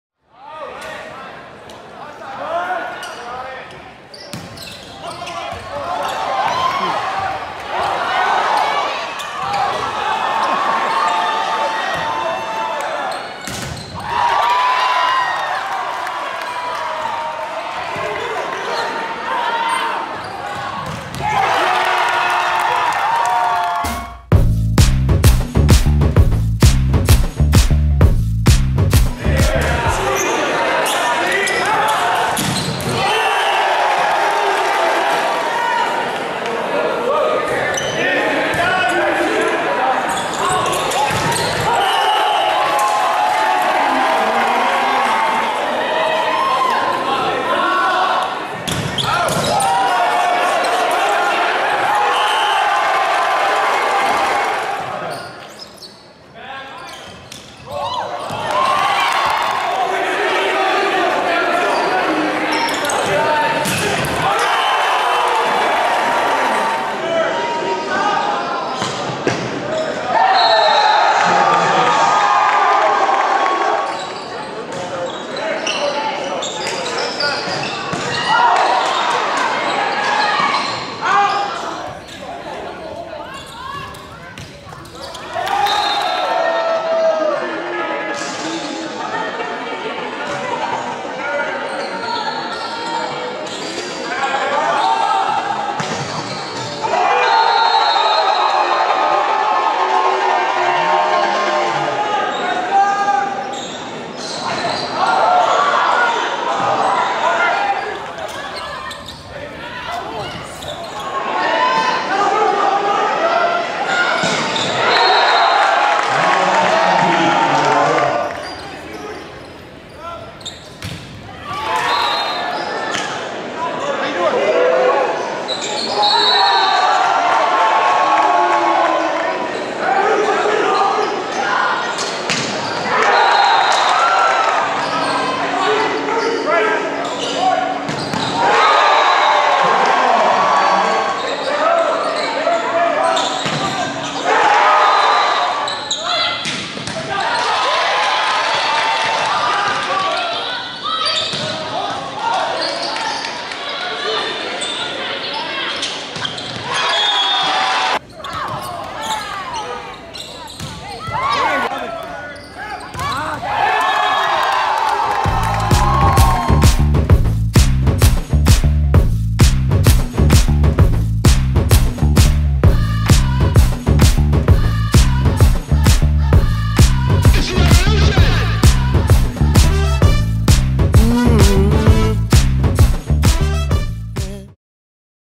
Colin Heath New Trier Volleyball 2019
Plays by Colin Heath in 2019 State of Illinois High School Volleyball Tournament